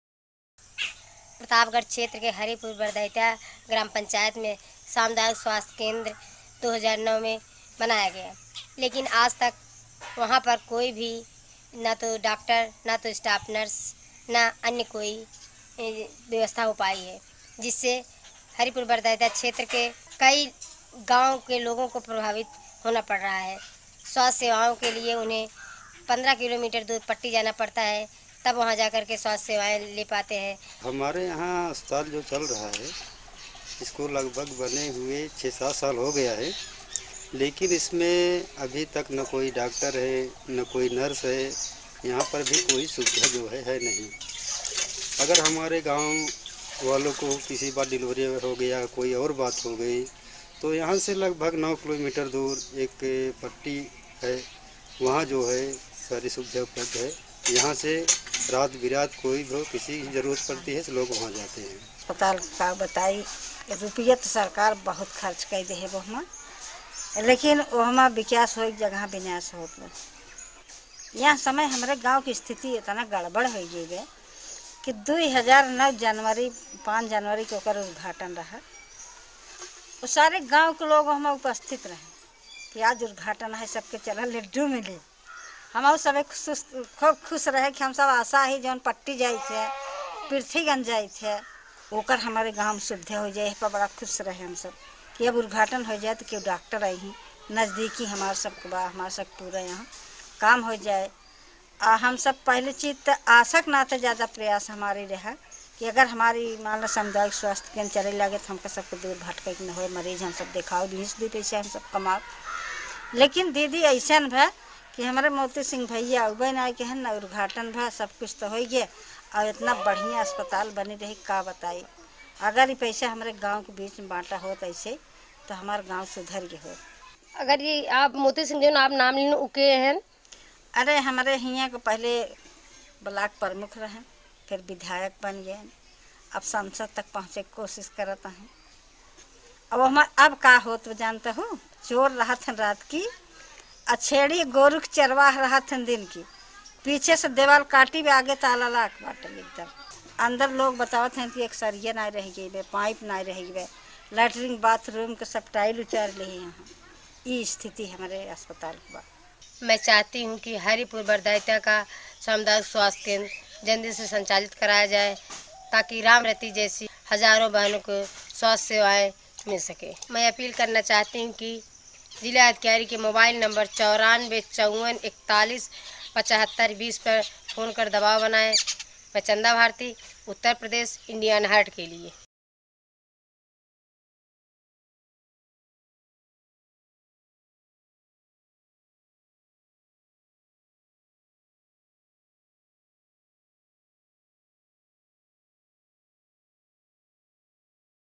Abandoned Hospital in Haripur Vardaita, Uttar Pradesh - Video Volunteer Chanda Bharti Reports
This PHC was built in 2009 but there have been no doctors, nurses or anybody else since and it lies in ruin.

This video was made by Video Volunteers Community Correspondent Chanda Bharti.